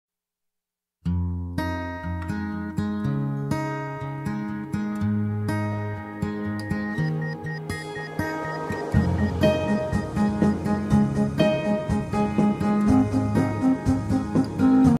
VASCA CON SPONDA MOBILE RAIPLAST
COLLAUDO VASCA PER UVA A TENUTA STAGNA CON SPONDA IDRAULICA